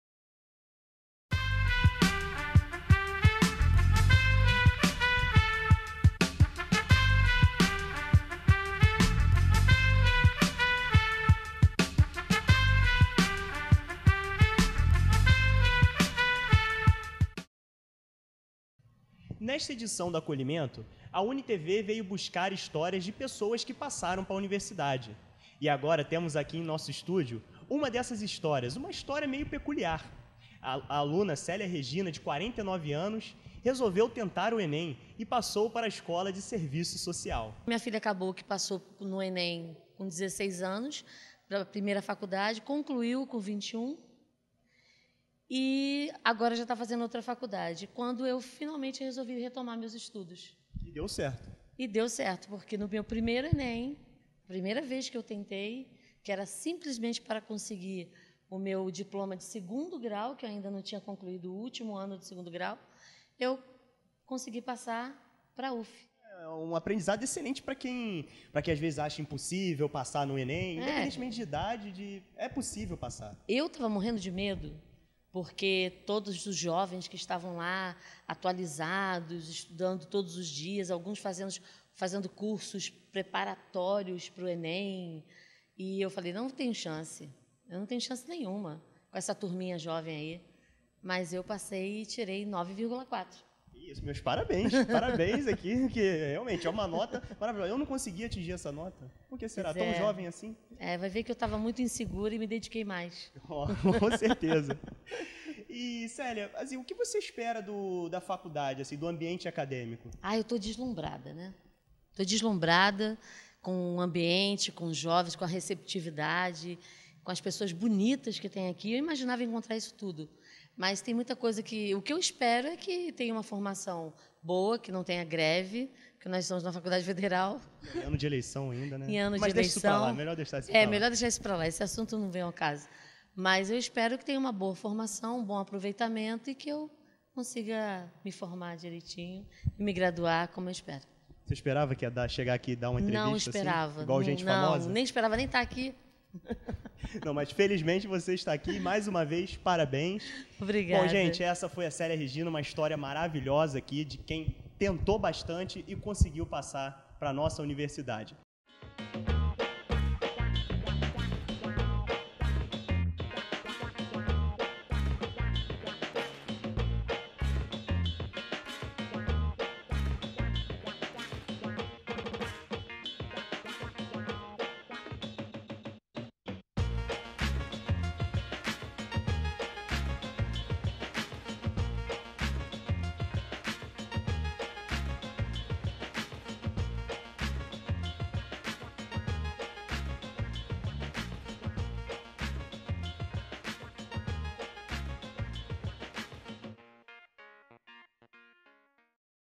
Programa Acolhimento Estudantil - Entrevista: Célia Regina
O Programa de Acolhimento Estudantil - PAE é um evento de recepção aos novos alunos que ingressam na UFF. Entrevista com Célia Regina, caloura de Serviço Social.

Campus do Gragoatá. 24/07/2014.

Entrevistada:
Célia Regina (Caloura de Serviço Social - UFF)

Produção:
Nadja Ribeiro

Jornalismo:
Pedro Henrique Lima

Direção:
Rodrigo Moraes

Coordenação geral:
Zé Luiz Sanz

Imagens:
Karina Uchôa
Ana Beatriz Bretas
Manoel Inácio

Mesa:
Jéssica Hartmann
Ariadne L. Zampaulo

Som:
Álvaro Luís

Transmissão:
Taiane Manhães
Rudolf Hans White van der Sluijs

Produção de estúdio:
Unirajara Marques dos santos Passos
Carlos Alberto Oliveira
Pedro Lima

Edição:
Ariadne L. Zampaulo

Unitevê - Agosto de 2014.